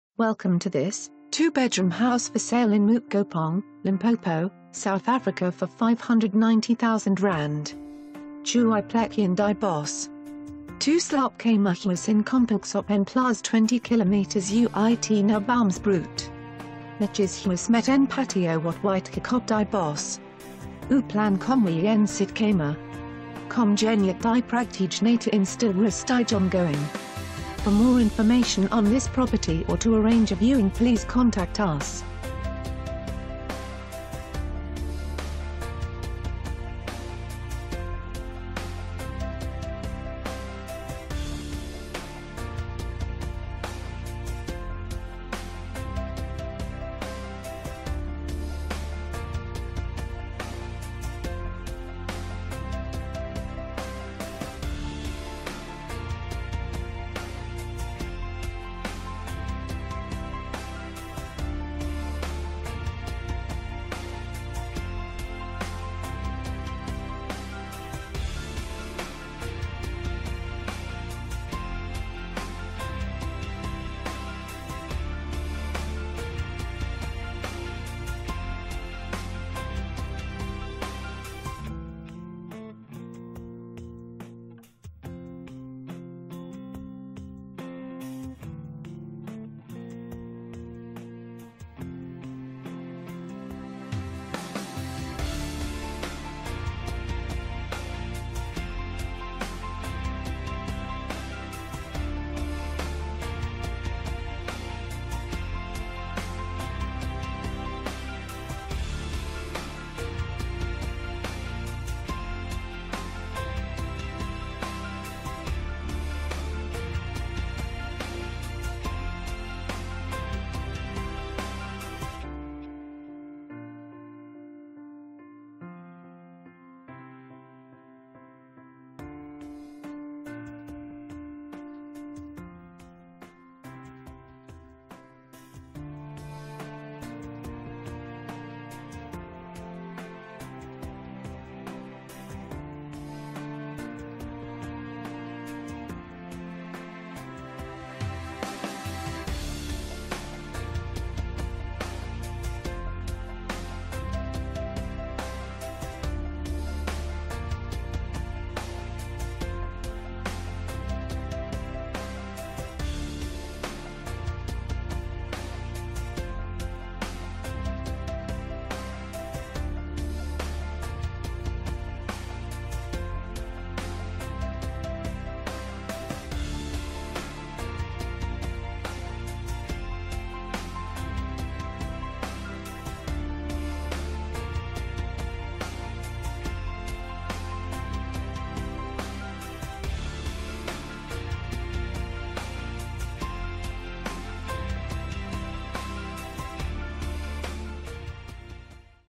2 Bedroom House For Sale in Mookgopong, Limpopo, South Africa for ZAR 590,000
Jou eie Plekkie in die Bos!
2 Slaap kamer huis in kompleks op n plaas 20 km uit Naboomspruit.
Netjiese huis met 'n patio wat uitkyk op die bos.
Oop plan kombuis en sit kamer.
Kom geniet die pragtige natuur in stil rustige omgewing!